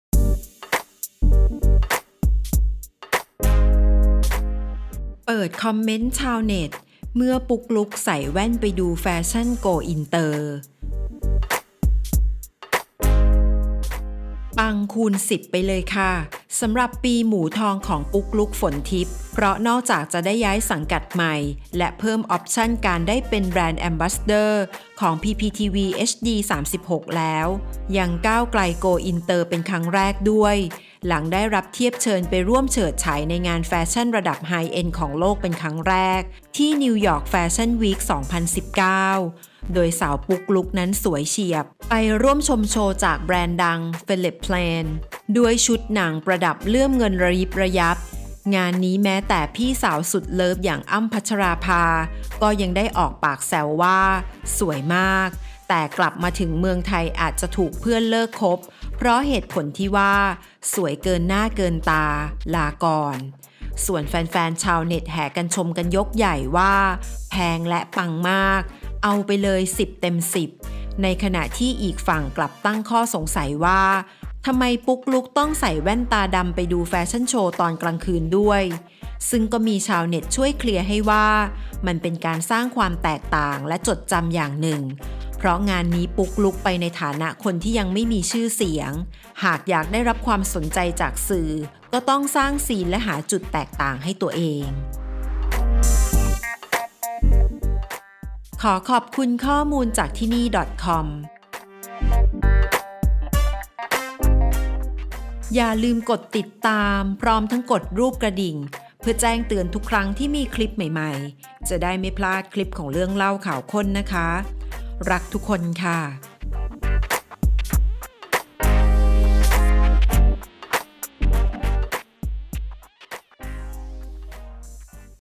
ไขข้อสงสัย ทำไมต้องใส่แว่น? เมื่อ ปุ๊กลุ๊ก ใส่แว่นไปดูแฟชั่นโกอินเตอร์
ปัง ปัง ปัง!! คูณสิบ ไปเลยจ้าา สำหรับปีหมูทอง ของ ปุ๊กลุ๊ก ฝนทิพย์ วัชรตระกูล เพราะนอกจากจะได้ย้ายสังกัดใหม่และเพิ่มออฟชั่นกับการได้เป็นแบรนด์แอมบาสเดอร์ ของ PPTVHD36 แล้วยังก้าวไกล โกอินเตอร์เป็นครั้งแรกด้วย หลังได้รับเทียบเชิญไปร่วมเฉิดฉายในงานแฟชั่นระดับไฮเอ็นท์ของโลกเป็นครั้งแรก ที่ นิวยอร์ค แฟชั่นวีก 2019